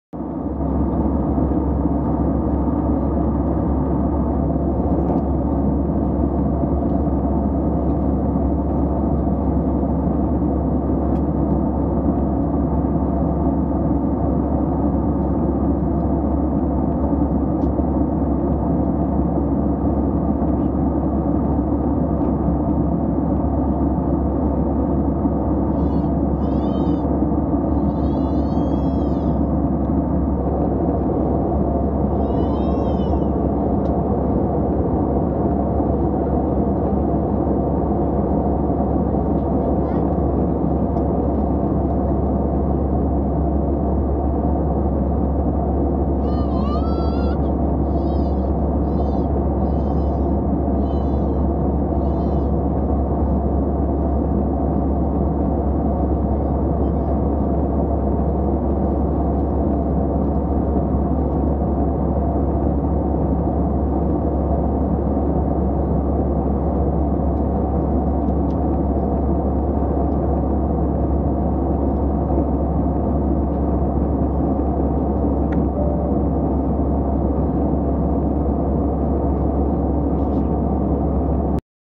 Google My tracks (failed to work) on airplane
The speed is so fast that my tracks can't work anymore. It's not able to graph the altitude too. Nothing is tracked except the elevation being displayed